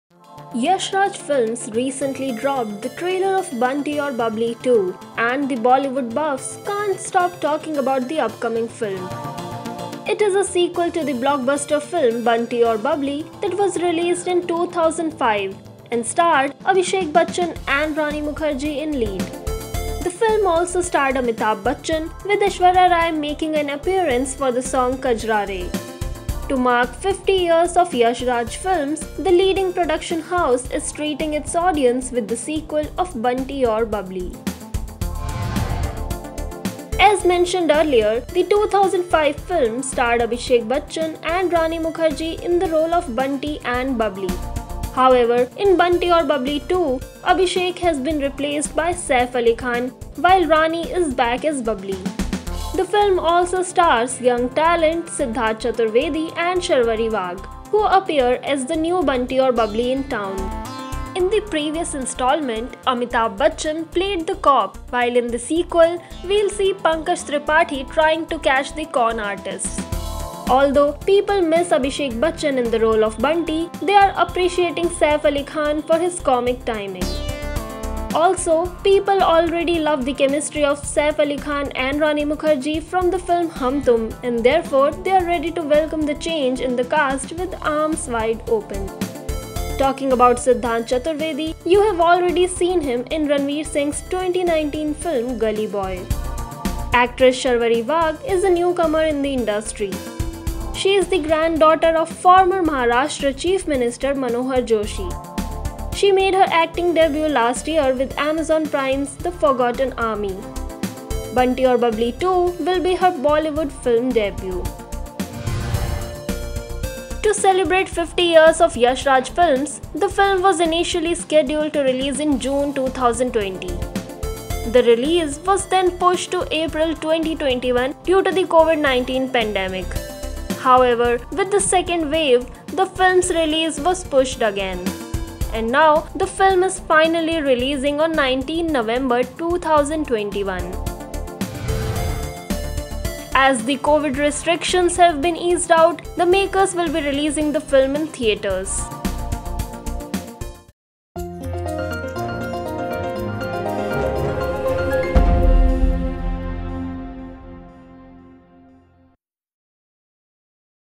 Bunty Aur Babli 2 Release Date, Cast, Where To Watch! Details Inside
Check out Bunty Aur Babli 2 release date, cast details and where to watch the film!